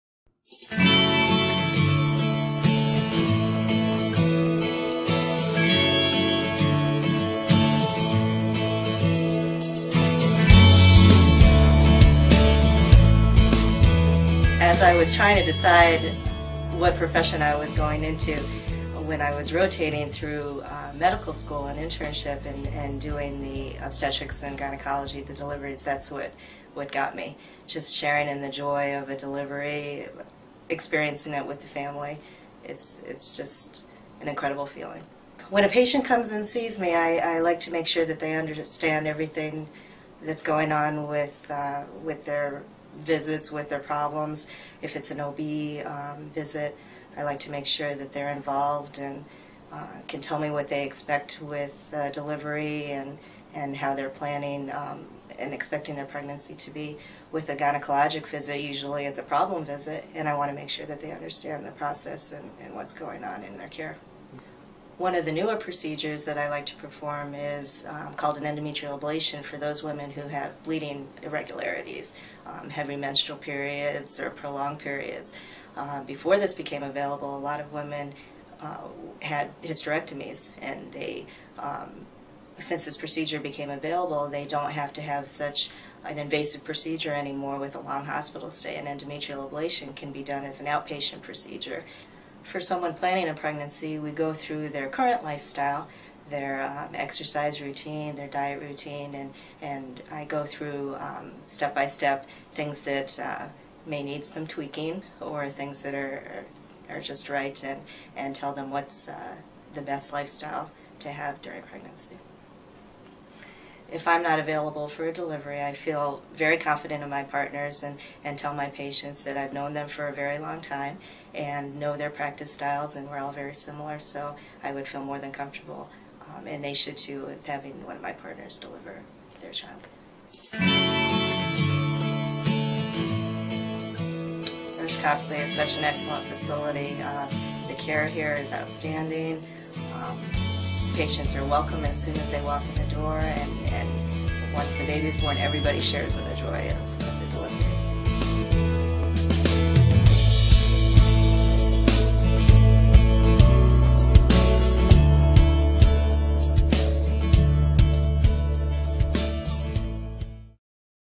Bernardita Druhan, DO, OB-GYN at RUSH
Bernardita Druhan, DO, is an OB-GYN with RUSH. In this video, Dr. Druhan discusses prioritizing patient understanding and involvement in their care, offering newer procedures like endometrial ablation for menstrual irregularities.